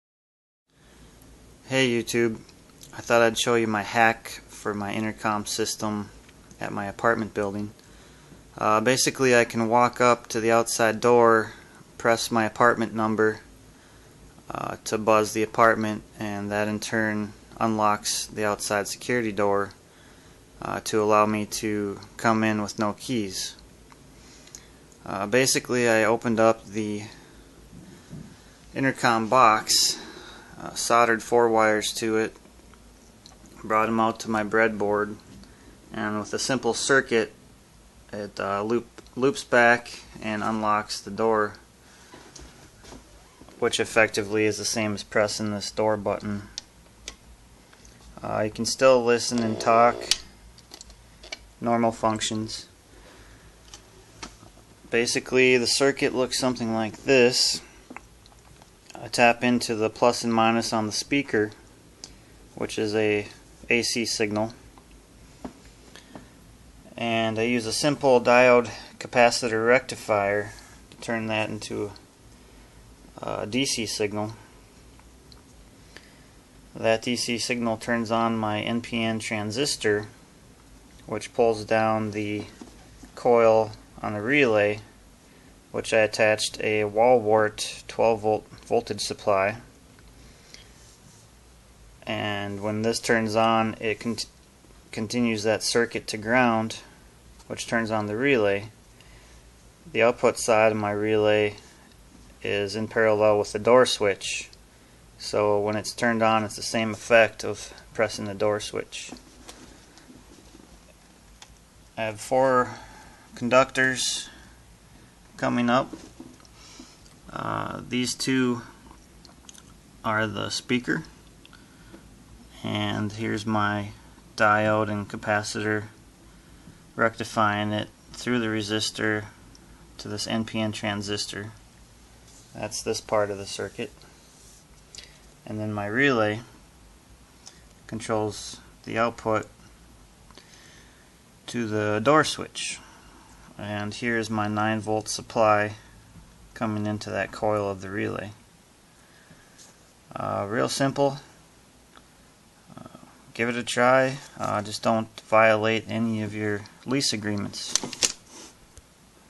Apartment Hack: Hacking Intercom for No-Key Door
This is a simple way to unlock your outside security door with no key! Pressing your apartment buzzer will unlock the door.Please note that this is not a very secure system, adding more circuitry to create a 'password' of Morse code would greatly improve security. I do not suggest anyone to violate their lease agreement. Please use this video as an instructional application of simple circuits.